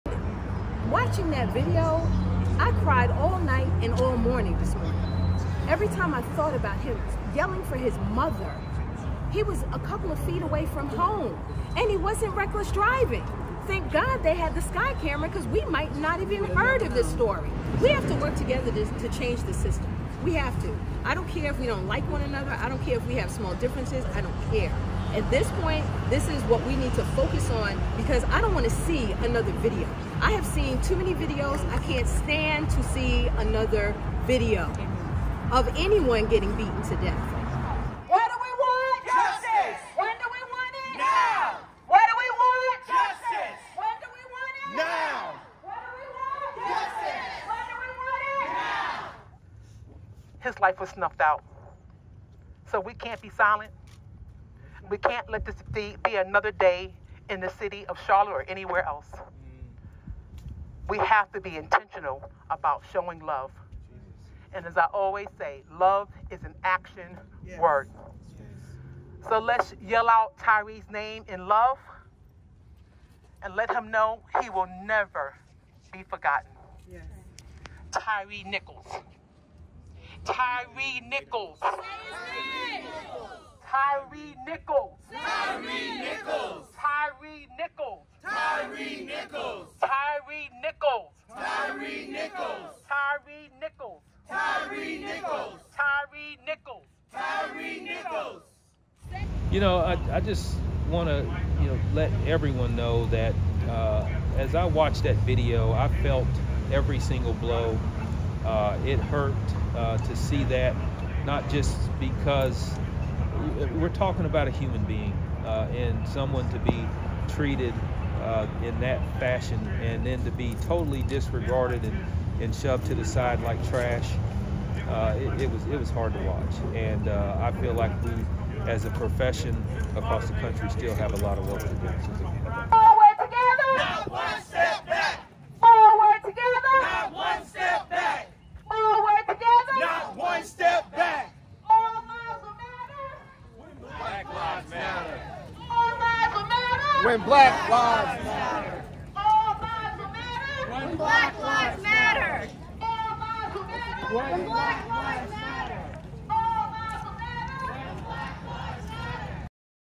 Charlotte Responds to Tyre Nichols' Death
Protesters took to the streets of Charlotte, North Carolina, on Saturday, Jan. 28, in response to the release of footage showing the brutal police killing of Tyre Nichols in Memphis.